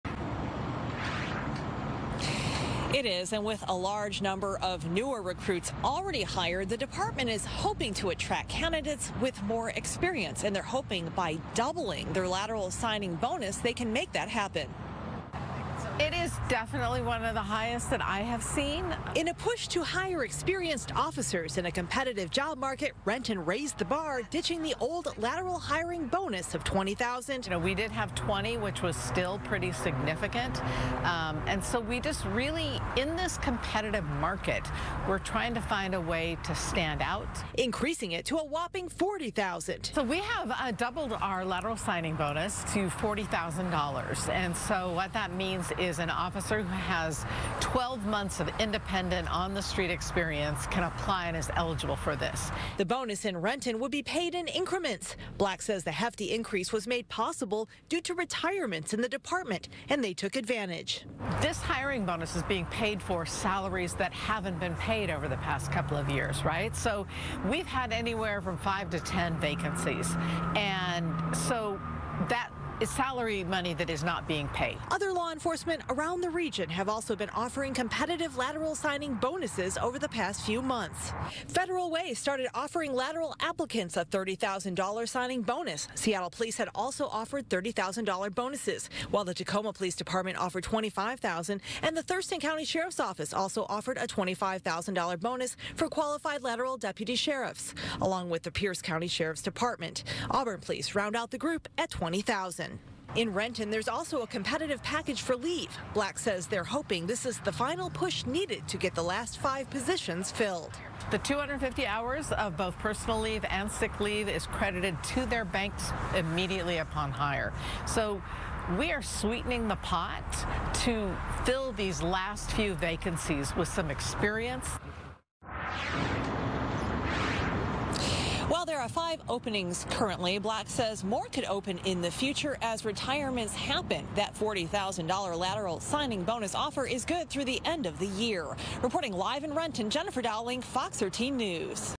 Renton Police offer 'one of the highest' signing bonuses | FOX 13 Seattle
The Renton Police Department is putting money where its mouth is to try and boost staffing levels at its department by having one of the highest lateral hiring bonuses in the area for experienced officers.